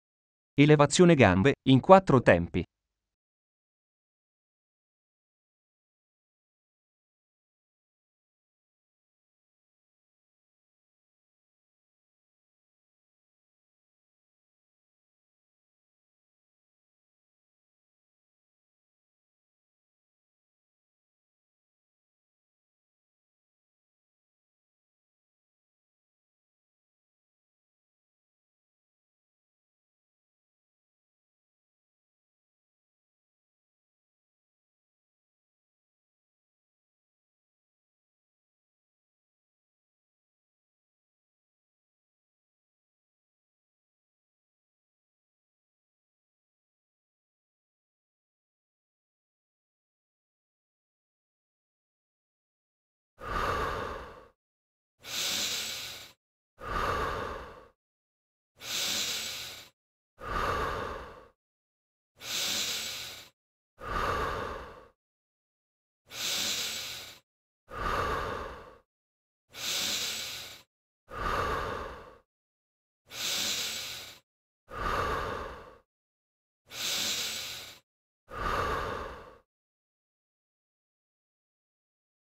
Addominali bassi elevazione in 4 tempi
Elevazione gambe in 4 tempi per lavorare gli "addominali bassi". Note allenamento addominali: è bene fare una precisazione riguardo l'allenamento di questo muscolo; il muscolo "retto dell'addome" è unico e vasto, spesso erroneamente suddiviso in muscoli addominali alti e muscoli addominali bassi. In realtà, queste specifiche , rappresentano solo delle aree del muscolo retto che possono essere stimolate con esercizi focalizzati ma che comunque coinvolgono sempre l'intero muscolo retto dell'addome.